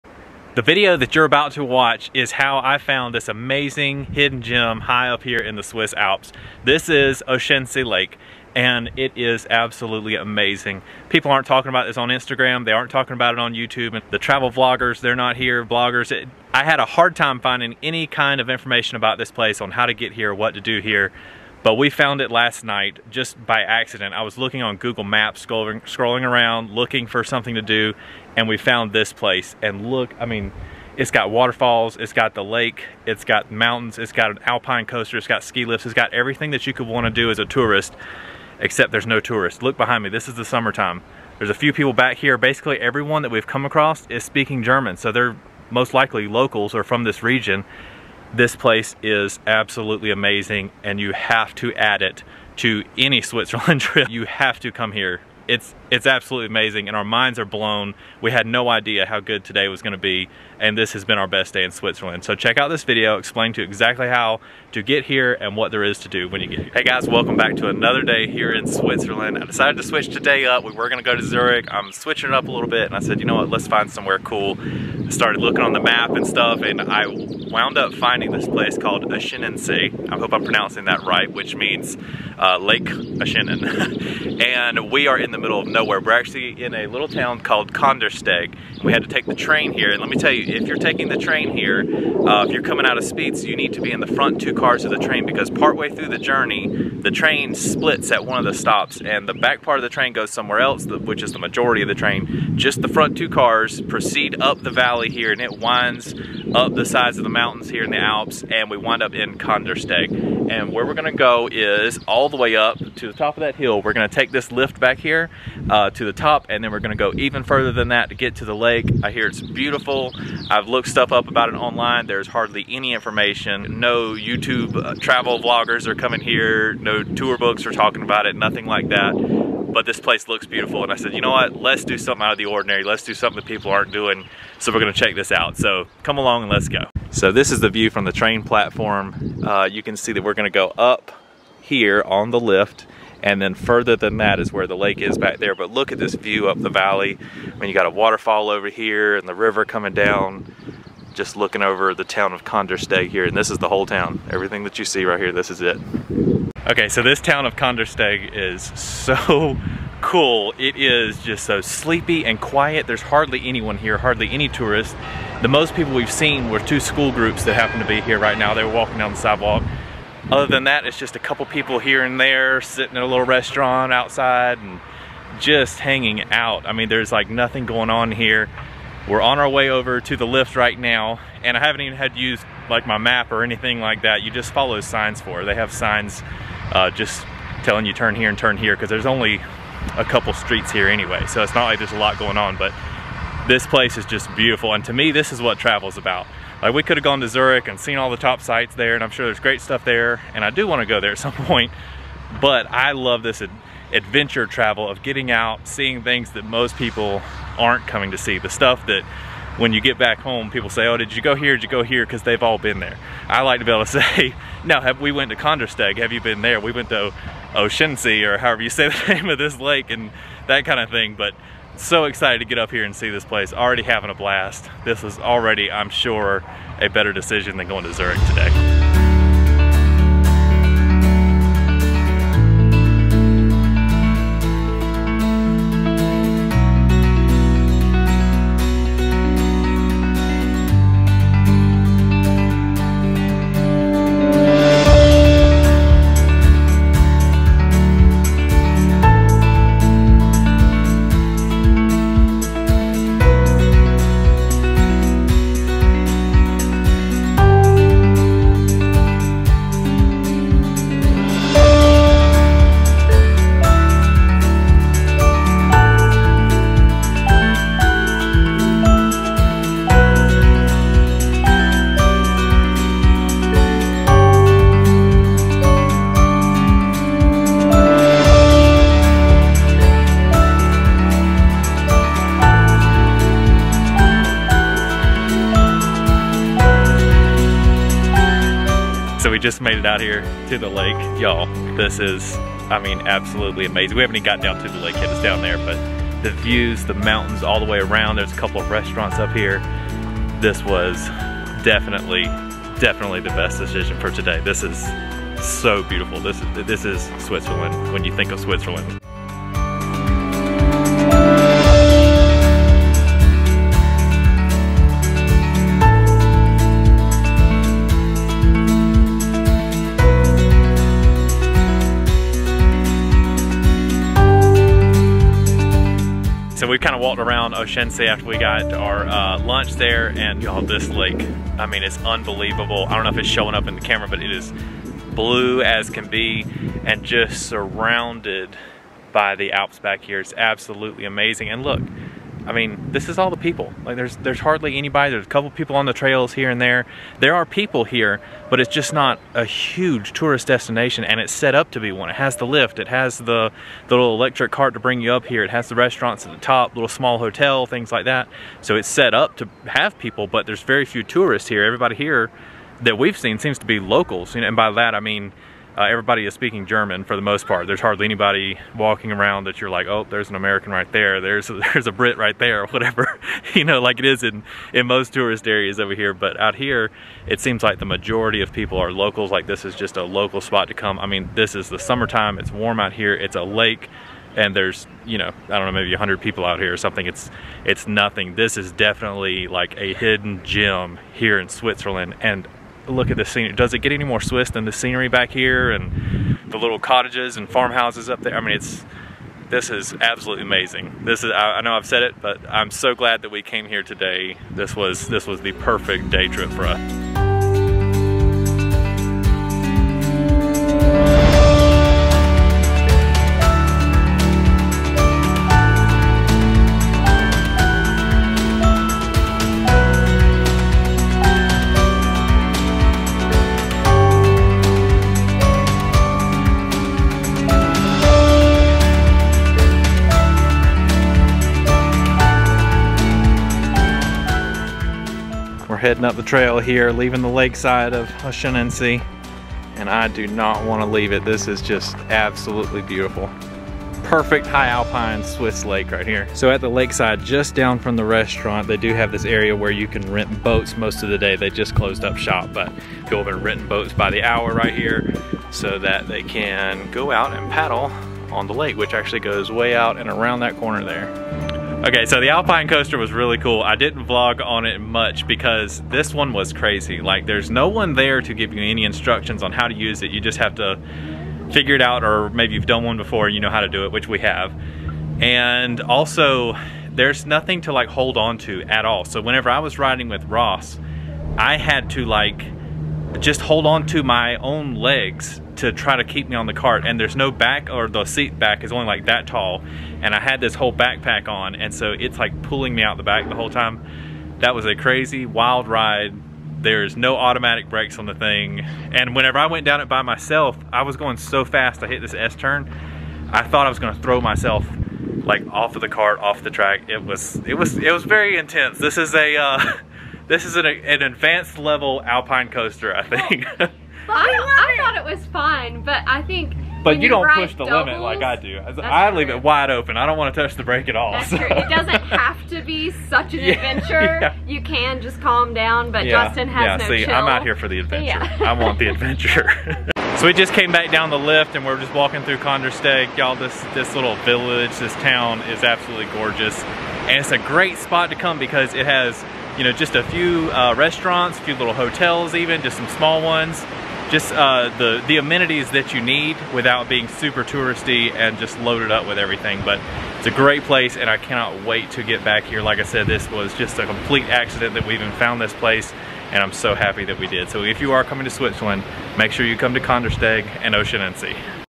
🇨🇭MUST SEE Hidden Gem in the Swiss Alps | Best Day in Switzerland 🏔
We found this place just by chance and we were surprised that more travelers aren't going there. This is a MUST SEE site in Switzerland. This turned out to be our favorite day in Switzerland! In this travel vlog, we will take you to Kandersteg, Switzerland to spend our day by Oscheninsee. This high-alpine lake is an amazing destination but it is just a bit tricky to get to and there is very little info about it online. Don't worry though, I'll show you exactly how to get there!

To travel with me or to have me book your trip,